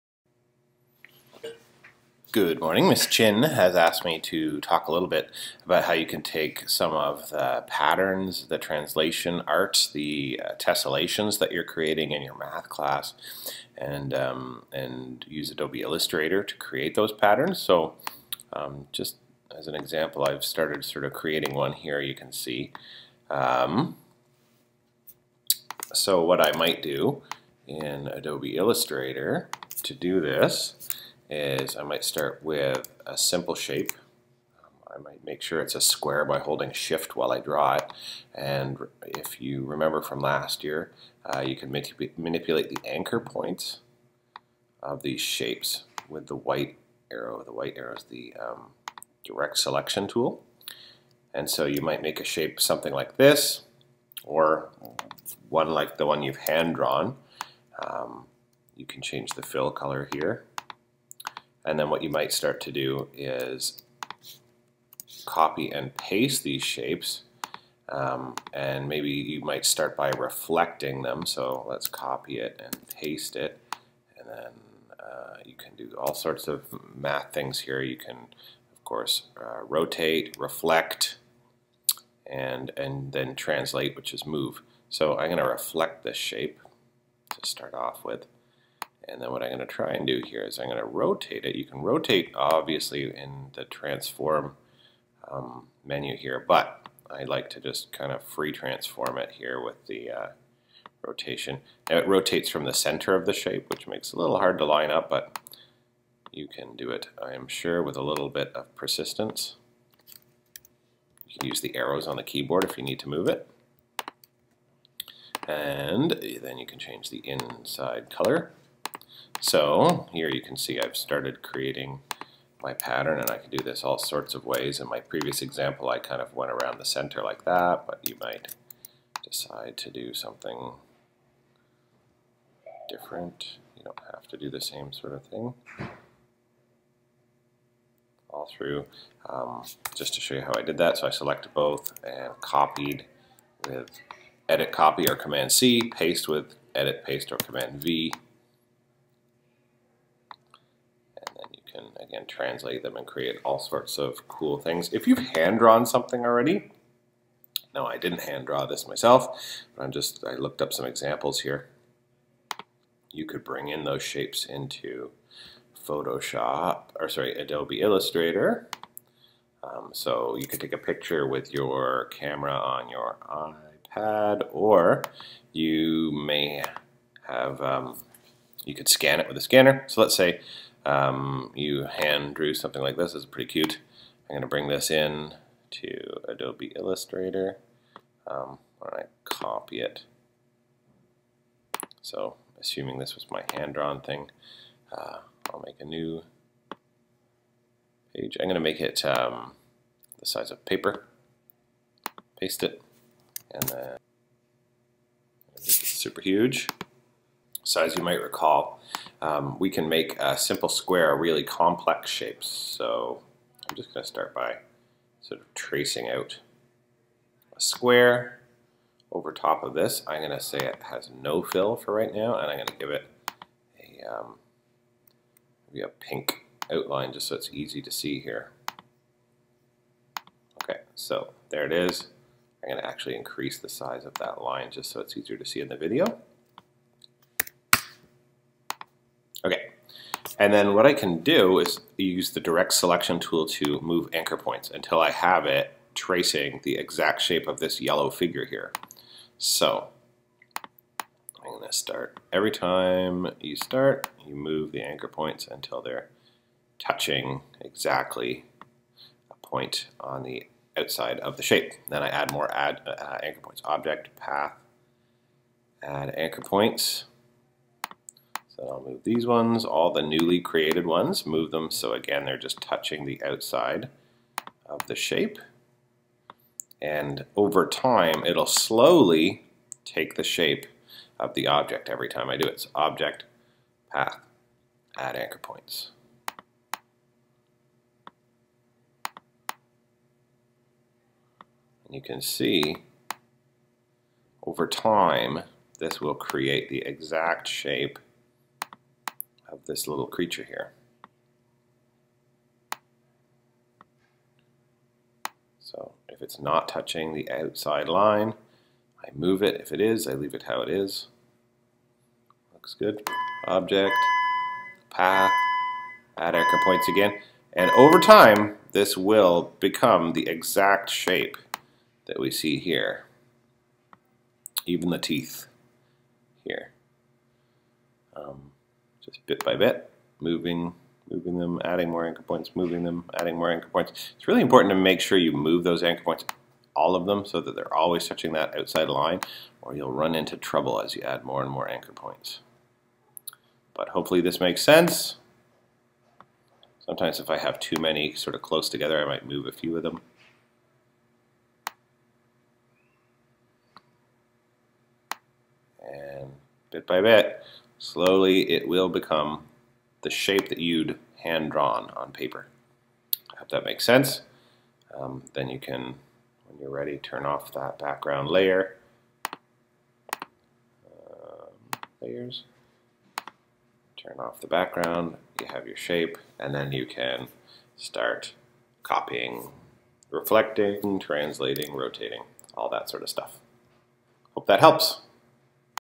Creating Translation Art / Tessellations in Adobe Illustrator
This video was created quickly to help one of our Math classes figure out how to create mathematical translation art, or tessellations using Adobe Illustrator.